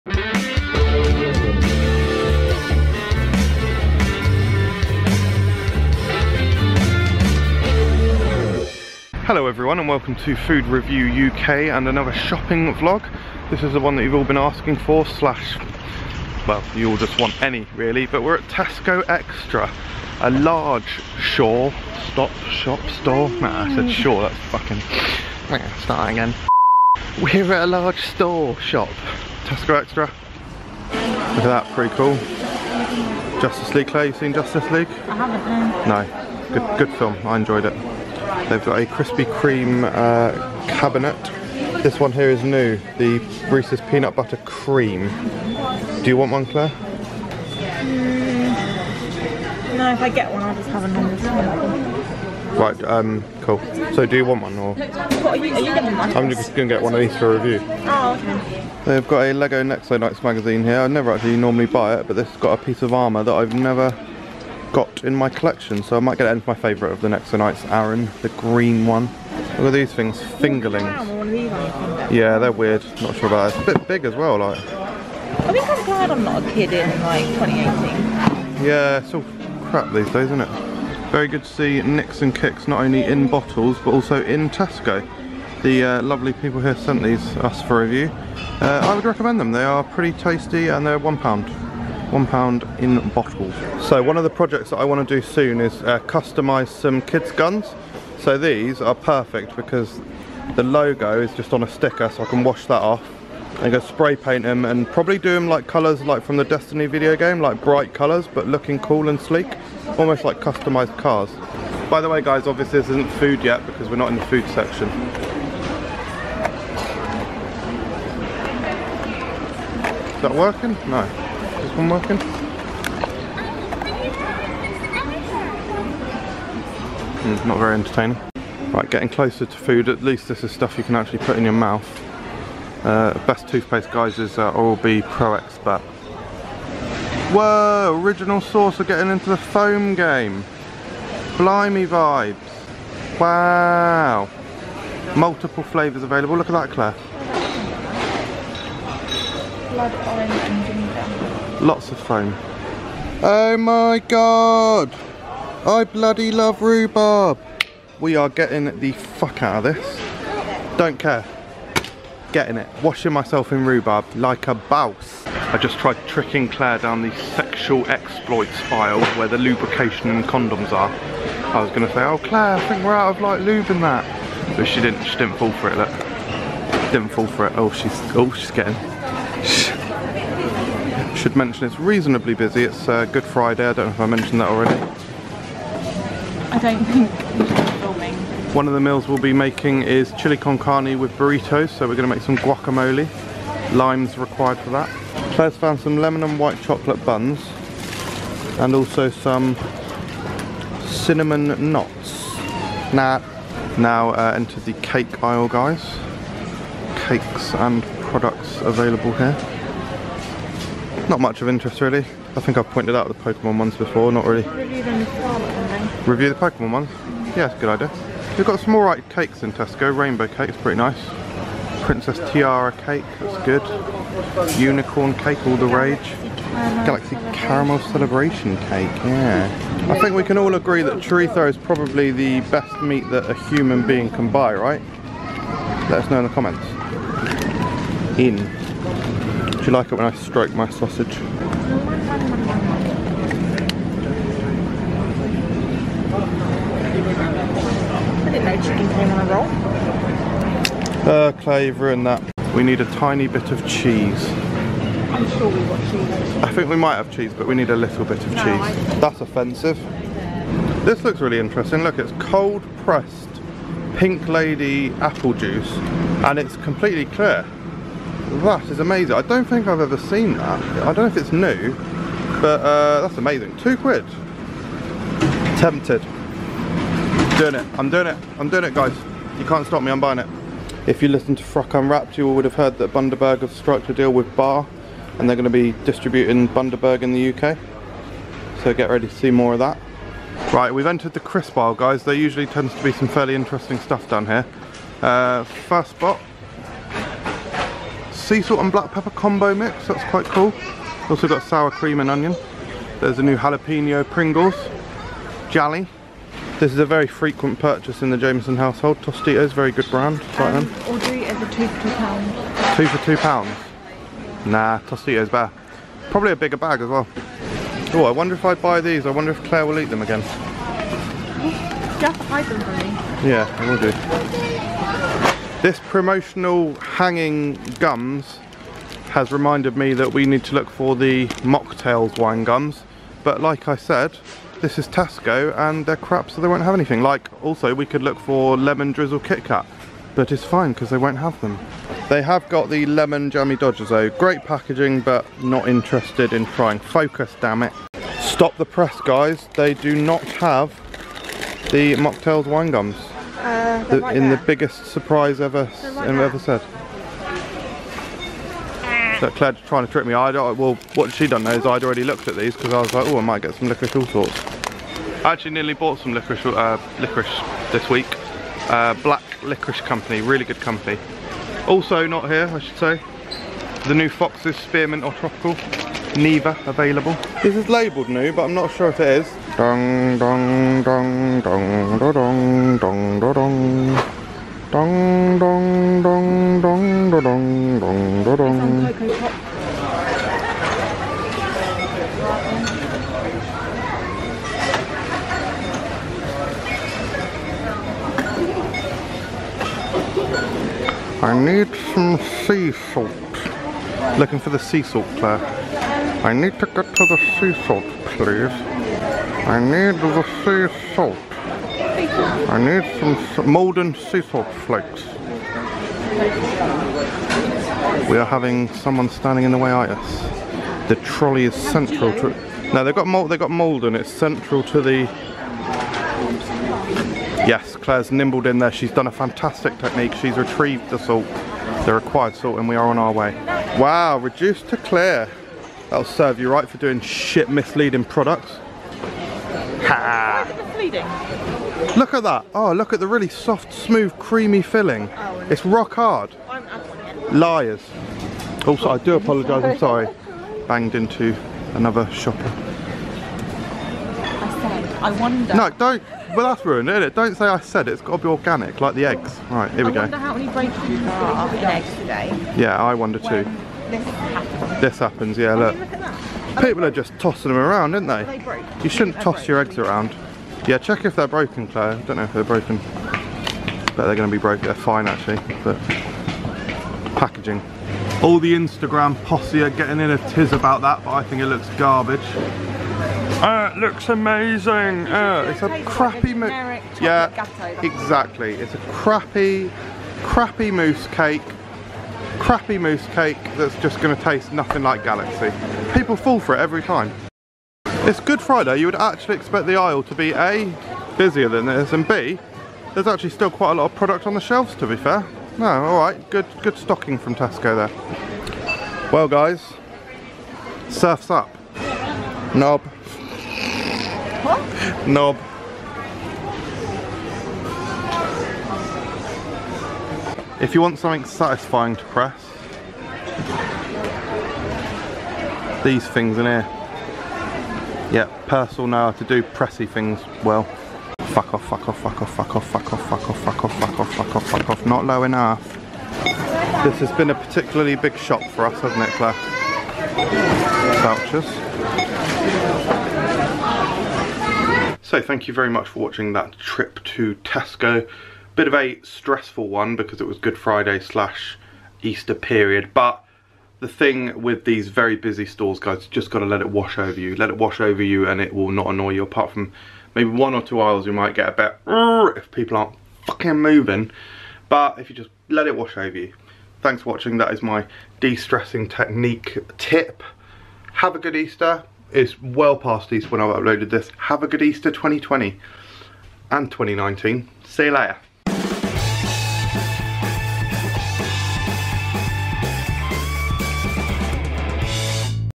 Tesco Shopping VLOG | April 2018 (Easter!)
MJ and Claire braved the mean streets of Aylesbury, and the popular Tesco Extra, a large version of Tesco and in fact the largest supermarket in Aylesbury. It was Good Friday and there were deals to be had...here we go.